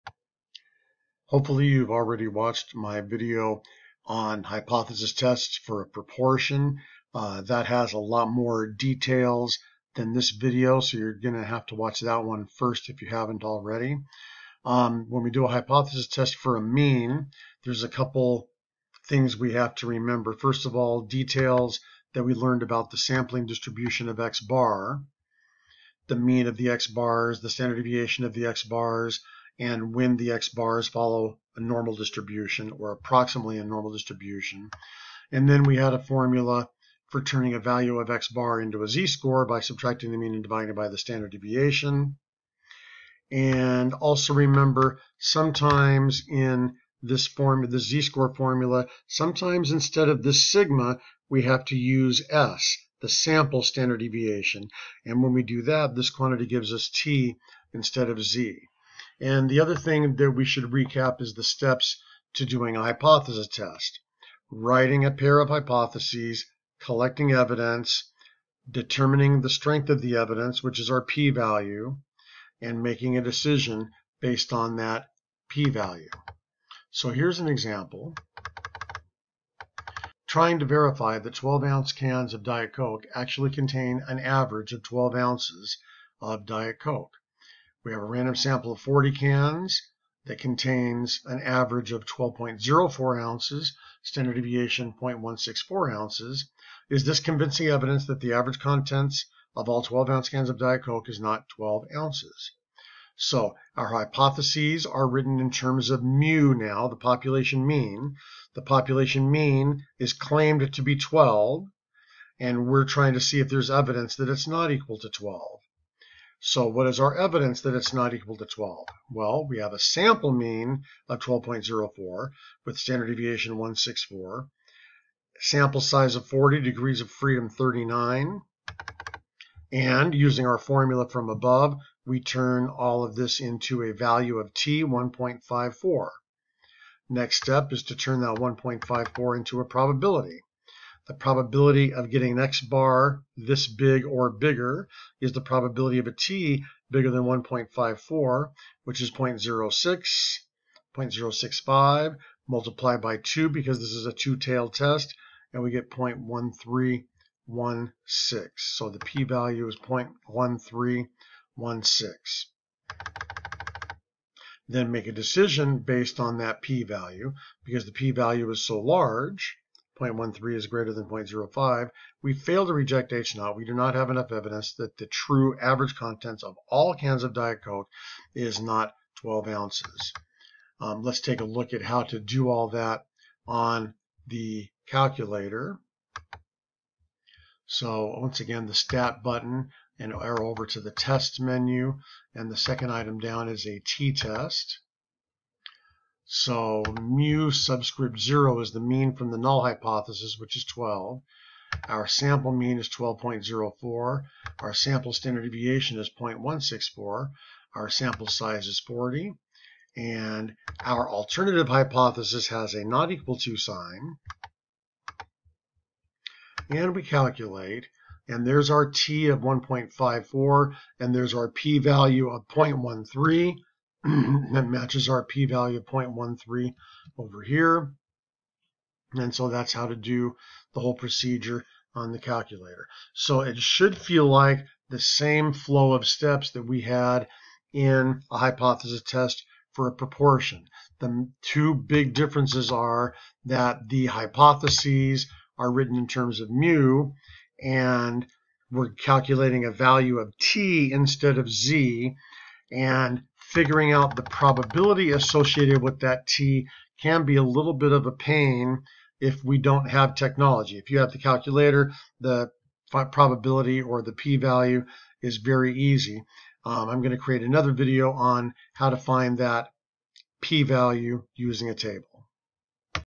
Hypothesis Test for a Mean
The instruction in this video assumes you have already seen my video and notes on hypothesis test for a proportion (which contains more details of a hypothesis test). This also shows how to do a hypothesis test for a mean (called a t test) on the TI-84 calculator.